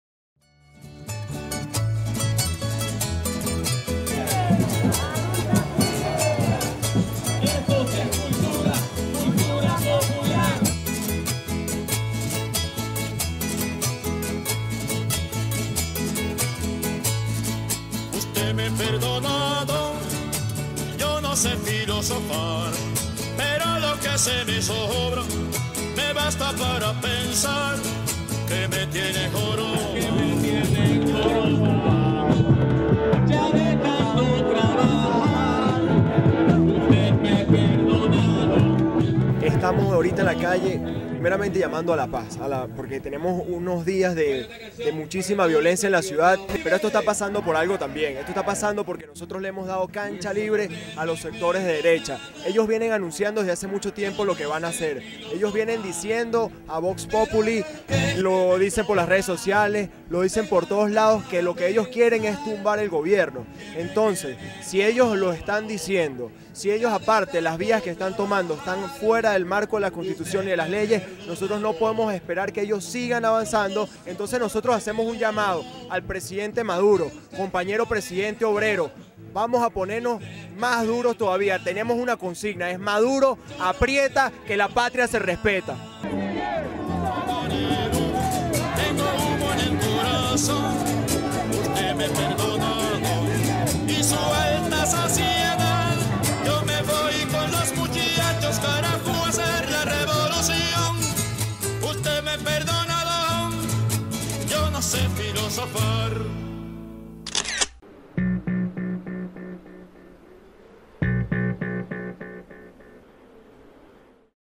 ¡Maduro aprieta que la Patria se respeta!
¡Maduro aprieta que la Patria se respeta! es la consigna que entonó la juventud merideña esta tarde en la Plaza Bolívar de Mérida, en apoyo al gobierno nacional. Tal como lo dijo el Comandante Che Guevara "al enemigo ni tantico así". No más pacto, no más negociación ni titubeos, necesitamos que el proceso revolucionario radicalice sus acciones y avance en la construcción de una sociedad nueva, llena de valores humanos donde la conciencia liberadora y la espiritualidad humanista guíe la edificación del hombre y la mujer nuevos.